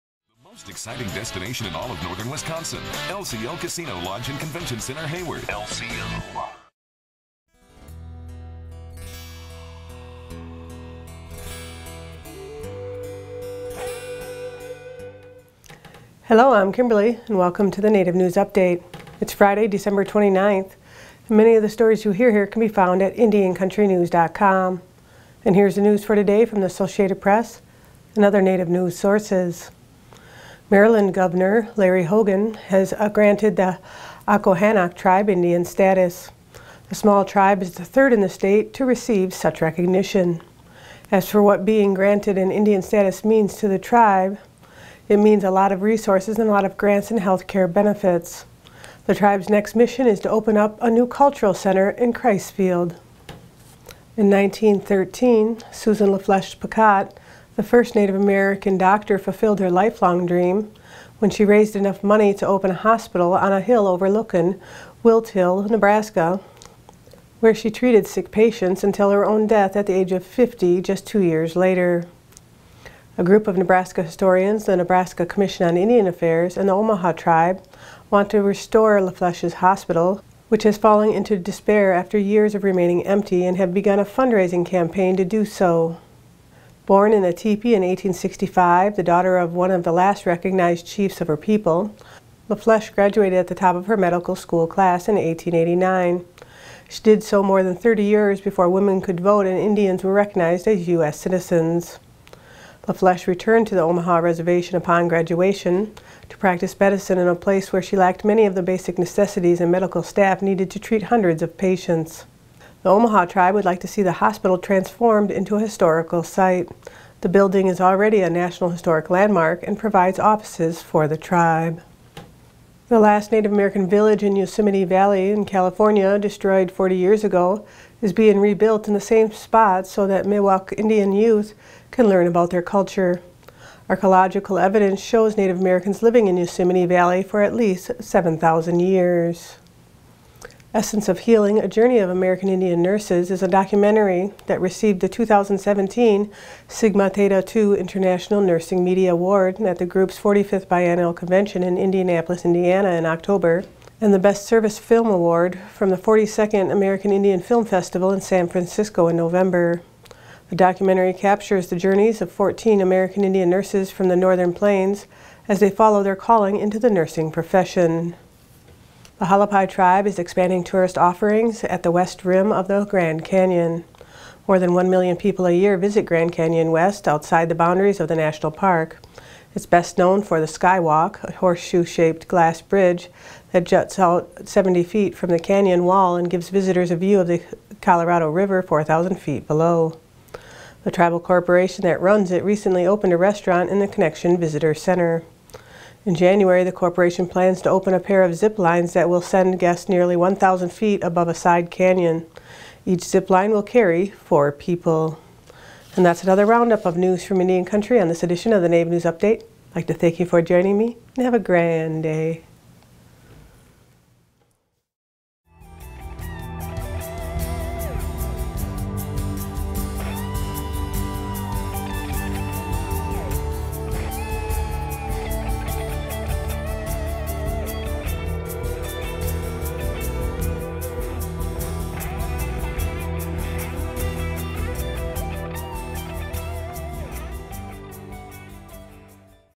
Native News Update December 29, 2017
This week's stories: The Accohannock Tribe receives state recognition; Omaha Tribe restoring hospital opened by the late Susan La Flesche Pecotte, the first Native American doctor; The last Native American village in Yosemite being rebuilt; Documentary on Native American nurses receives two awards.

Native News Update with anchor Kimberlie Acosta from the studios of IndianCountryTV.com